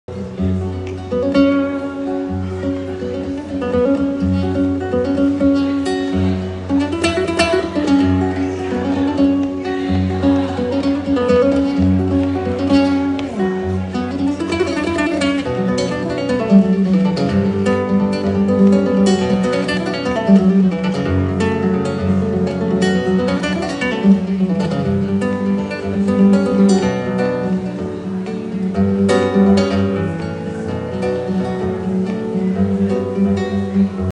Tidal Pools 6/1/22
Short clip from a solo performance at South & Central restaurant on 6/1/22.